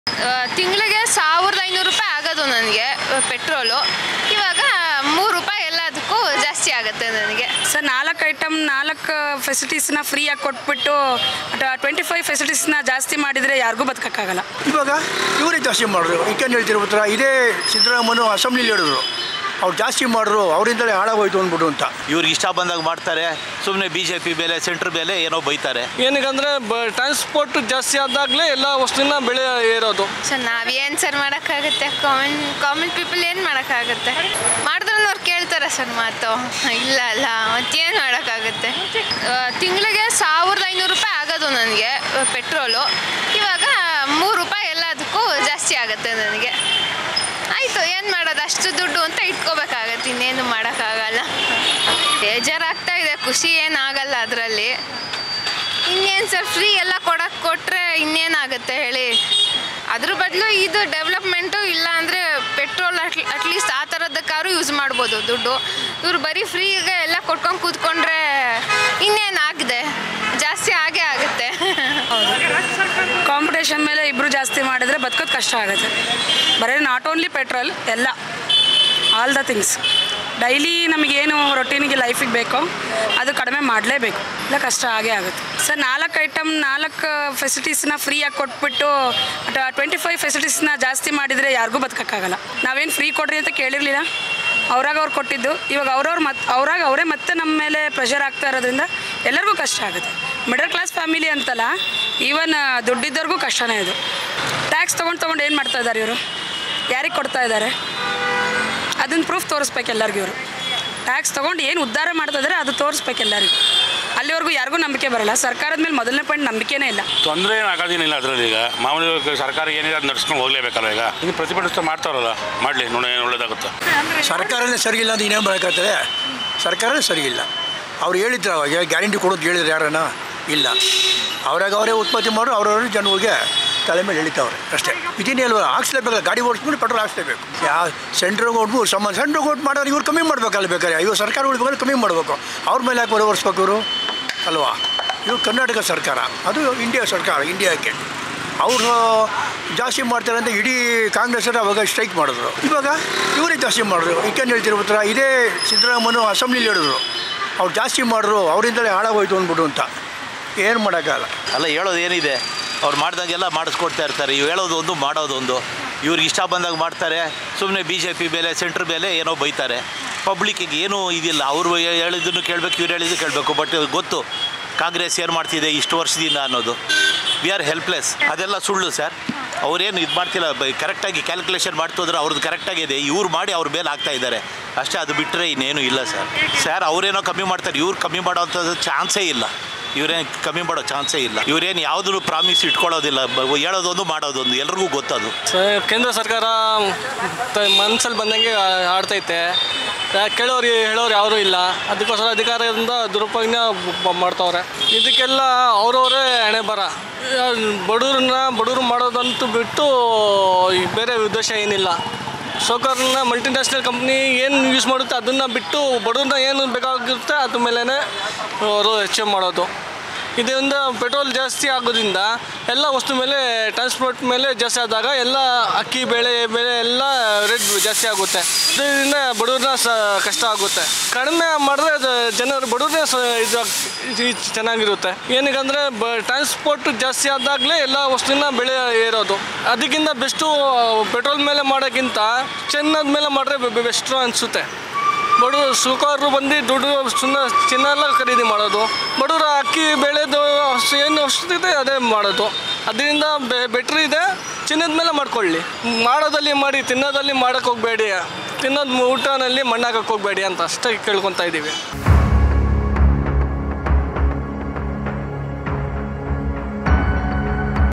"ನಾಲ್ಕು ವಸ್ತು ಫ್ರೀ ಕೊಟ್ಟು, 25 ವಸ್ತುಗಳಿಗೆ ಬೆಲೆ ಏರಿಕೆ ಮಾಡಿದ್ರೆ ಬದುಕೋದು ಹೇಗೆ?.." | Karnataka
"ಆಗ ಪ್ರತಿಭಟನೆ ಮಾಡಿದ್ದ ಸಿದ್ದರಾಮಯ್ಯ ಅವರೇ ಈಗ ಜಾಸ್ತಿ ಮಾಡಿದ್ರು?.."

► "ಸರಕಾರ ನಡೆಸಲು ಪೆಟ್ರೋಲ್, ಡೀಸೆಲ್ ಬೆಲೆ ಹೆಚ್ಚು ಮಾಡಲೇ ಬೇಕಲ್ವಾ.."

► "ಉಚಿತ ಗ್ಯಾರಂಟಿ ಕೊಟ್ಟು ಪೆಟ್ರೋಲ್, ಡಿಸೇಲ್ ಬೆಲೆ ಜಾಸ್ತಿ ಮಾಡಿದ್ದಾರೆ..."

► ಪೆಟ್ರೋಲ್, ಡೀಸೆಲ್ ಬೆಲೆ ಏರಿಕೆ ಬಗ್ಗೆ ಬೆಂಗಳೂರಿನ ಜನರ ಪ್ರತಿಕ್ರಿಯೆ